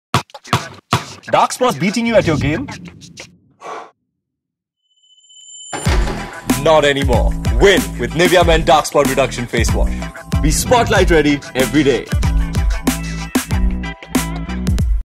Ready for the Spotlight: NIVEA Men's Dark Spot Reduction Face Wash with Ranveer Singh | Schbang
Be spotlight ready with Nivea Men
Film conceptualised and created by Schbang Motion Pictures